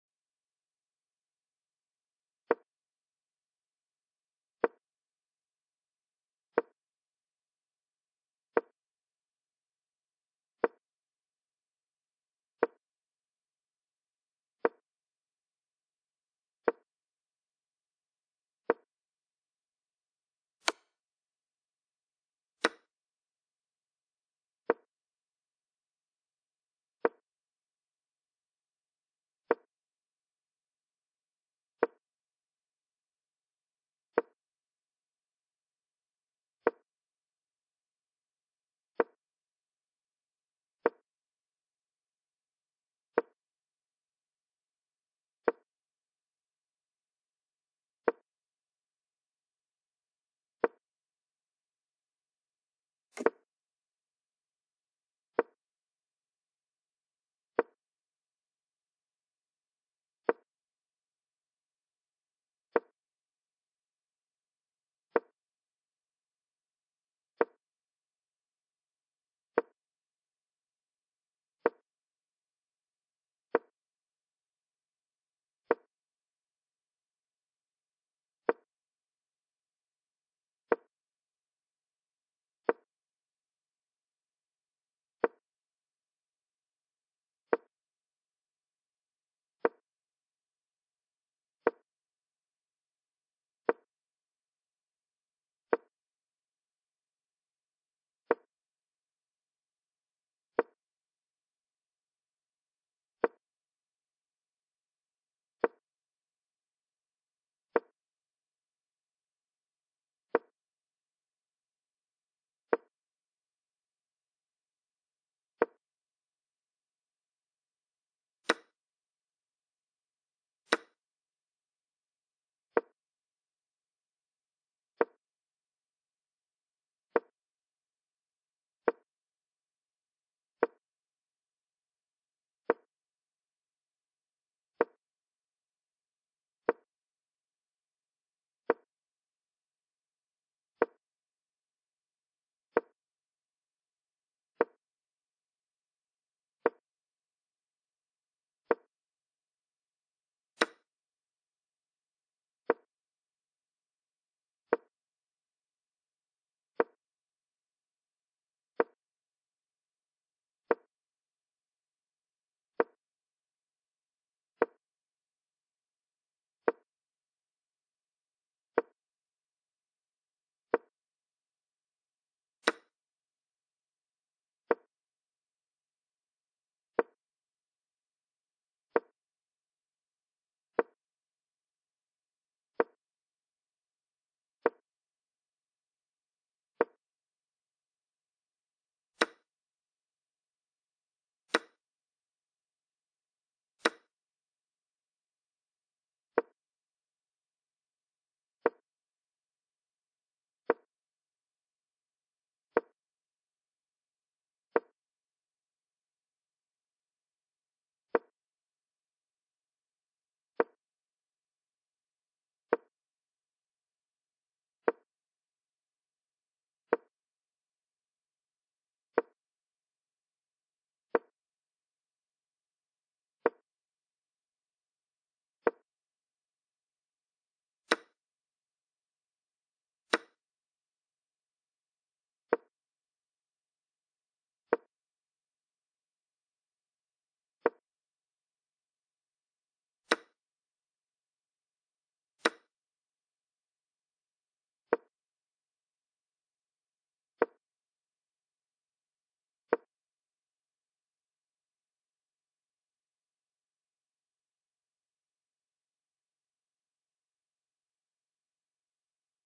GM Arjun Erigaisi vs. GM Sanan Sjugirov | Chennai Grand Master 2023 | Round 7
Arjun fired on all cylinders to play a complicated game even with the tournament title at stake and defeated GM Sanan Sjugirov in a determined effort. Gukesh thus moves closer to earning a berth at the Candidates Tournament 2024.